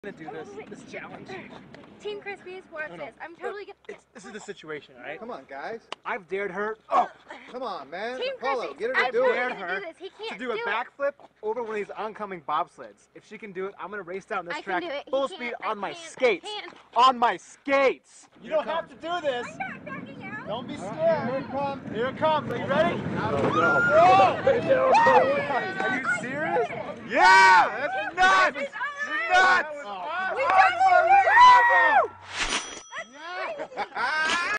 Shawn Johnson backflip over a Bobsled
Shawn gettin crazy